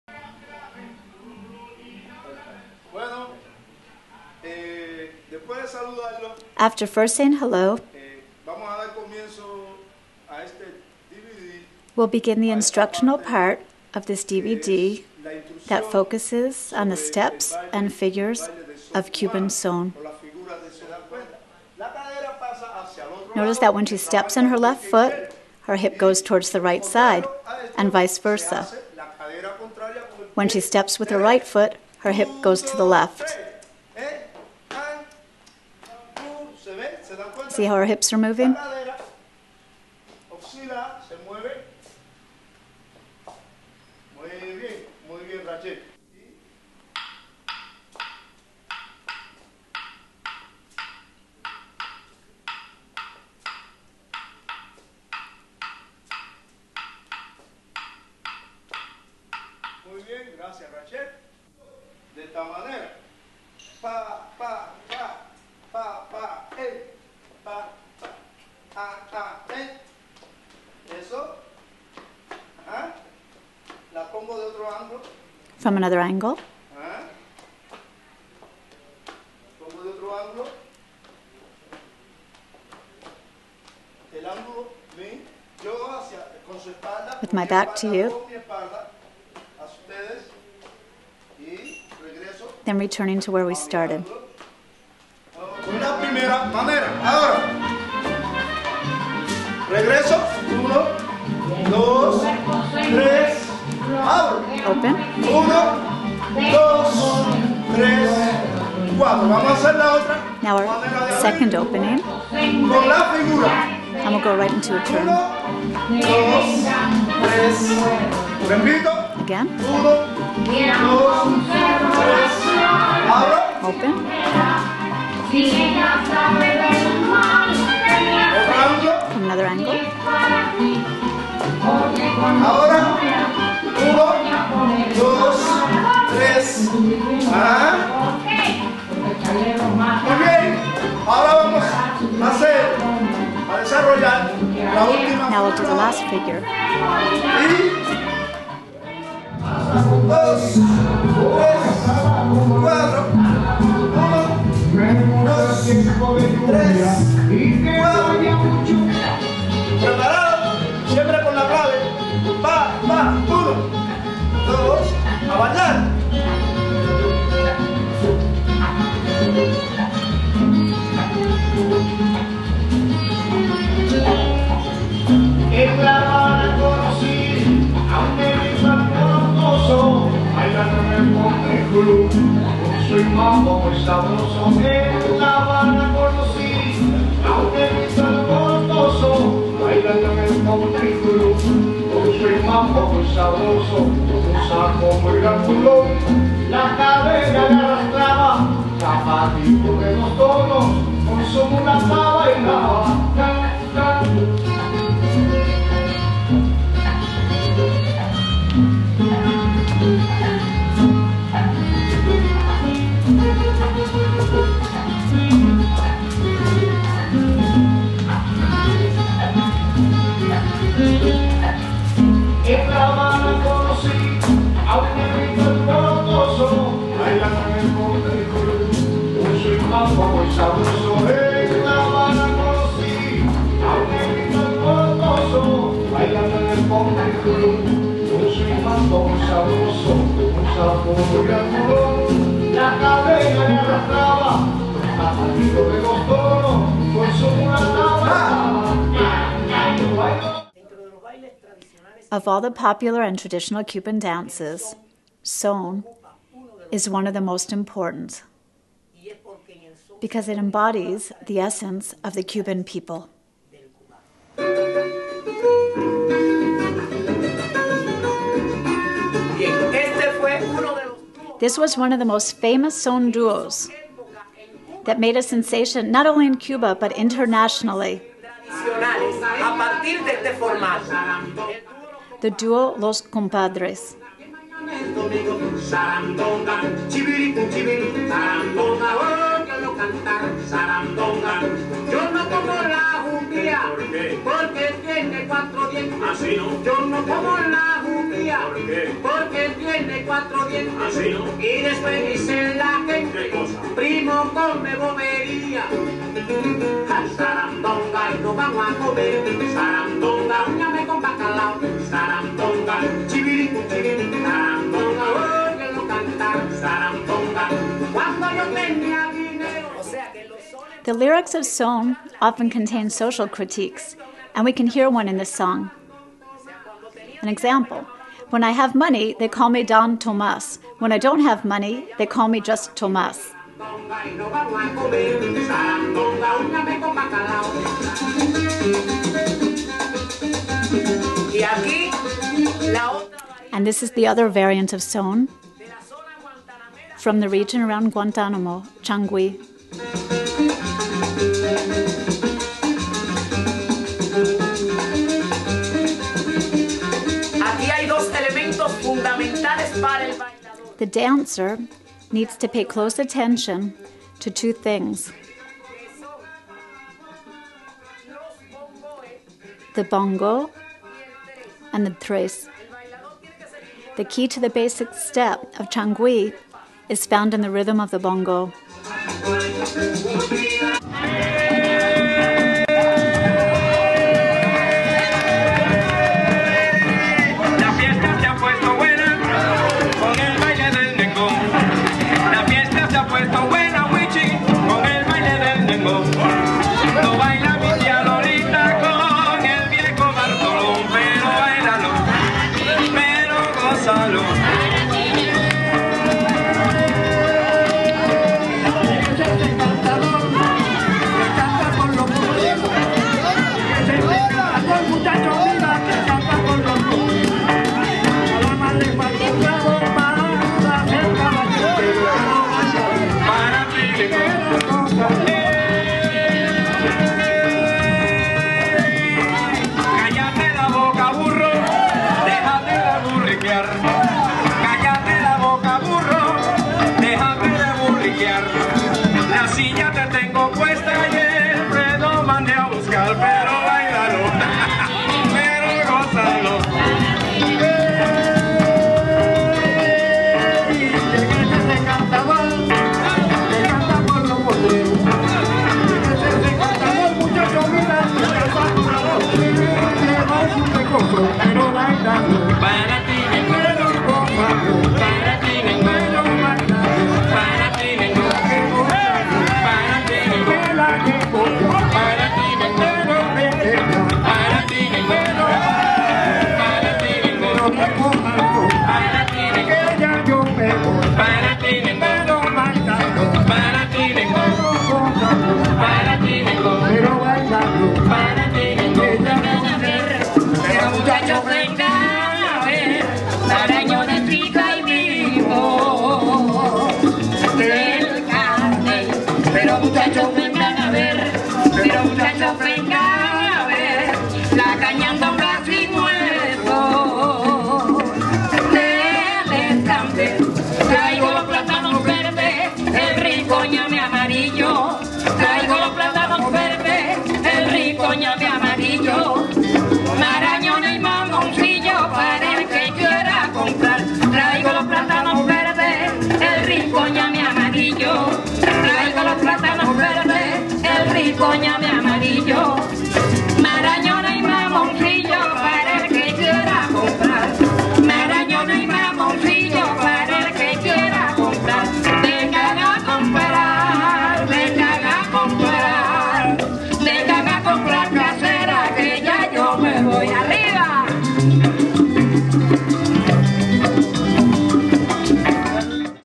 SON CUBANO - Instructional Cuban dance DVD featuring Jose Carrion
SON CUBANO is the 3rd in a series of instructional Cuban dance DVDs to be produced by the Academy of Cuban Folklore and Dance. This DVD features detailed instruction on Cuban Son including the basic step with clave, turn combinations & tornillos.  The DVD also includes a lecture, "Raices del Son", as well as demos & performances by Cutumba.

Dentro de la cultura Cubana, el Son es uno de los bailes de mayor arraigo, teniendo como regla las figuras que pertenecen a esta manifestación solamente. En este DVD, les brindamos la posibilidad de aprender y desarrollar, de forma teórica y práctica, todo lo relacionado a los diferentes formatos músicales y coreográficos que contribuyen al Son Cubano. Esta va dirigido a todos los amantes del Son Cubano, ya sean principiantes, intermedios y avanzados.

In Spanish, with English voice-over option. Filmed in Santiago de Cuba, 2009.

José Carrión was the principal dancer & corps professor of Ballet Folklórico Cutumba de Santiago de Cuba. After 25 years of teaching & presenting both folkloric & popular Cuban dance, José retired from Cutumba. He now lives in the US, where he co-directs the Academy of Cuban Folkore & Dance in Seattle, Washington.